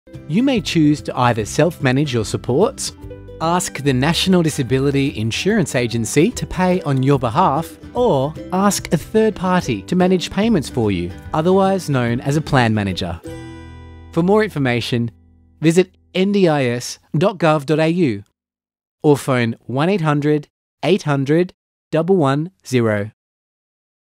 How To - Ways to manage your funding
Our Participant Service Guarantee provides us with clear direction to improve your experience with the NDIS.

The legislated Participant Service Guarantee provides timeframes for key NDIS processes.